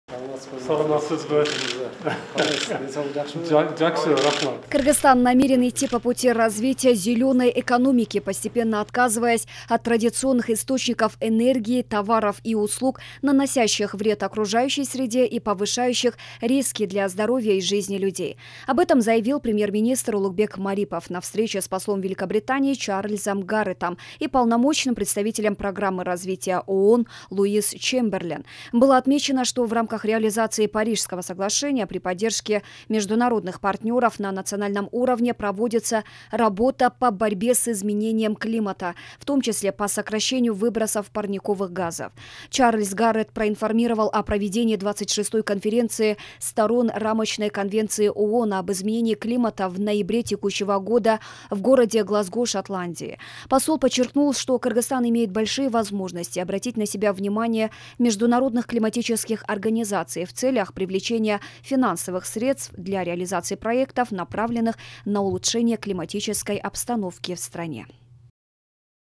Кыргызстан намерен идти по пути развития «зелёной» экономики
Кыргызстан намерен идти по пути развития «зелёной» экономики, постепенно отказываясь от традиционных источников энергии, товаров и услуг, наносящих вред окружающей среде и повышающих риски для здоровья и жизни людей. Об этом заявил Премьер-министр Улукбек Марипов на встрече с послом Великобритании Чарльзом Гарреттом и Полномочным представителем Программы развития ООН Луиз Чемберлен.

Было отмечено, что в рамках реализации Парижского соглашения при поддержке международных партнеров на национальном уровне проводится работа по борьбе с изменением климата, в том числе по сокращению выбросов парниковых газов. Чарльз Гарретт проинформировал о проведении 26-ой Конференции Сторон Рамочной конвенции ООН об изменении климата в ноябре текущего года в городе Глазго, Шотландия. Посол подчеркнул, что Кыргызстан имеет большие возможности обратить на себя внимание международных климатических организаций в целях привлечения финансовых средств для реализации проектов, направленных на улучшение климатической обстановки в республике.